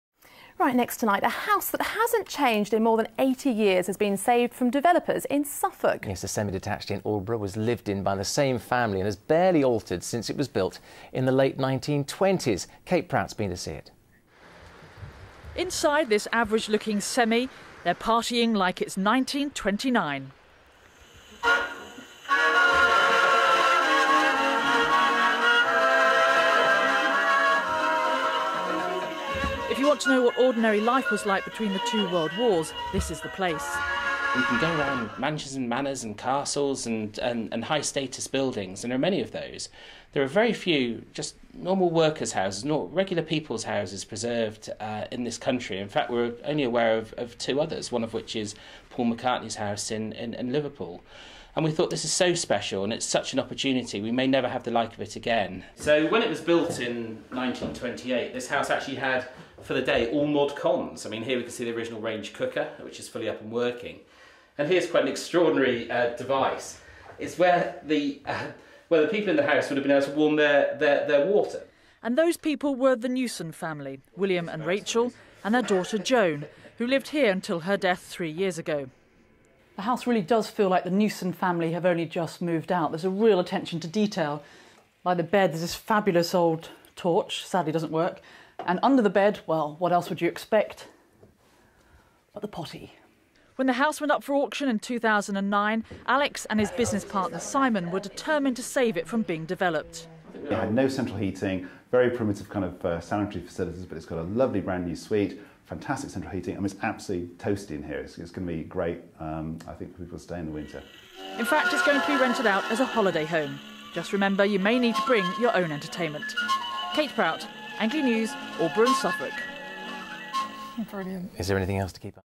Best of Suffolk - The 1928 House - ITV Anglia
On the day Best of Suffolk launched the 1928 House, ITV Anglia came to visit the property and spoke to director Alex Tarry and friend Simon James about their careful restoration of a truly special property. To find out more about this unique, luxury holiday cottage in Aldeburgh,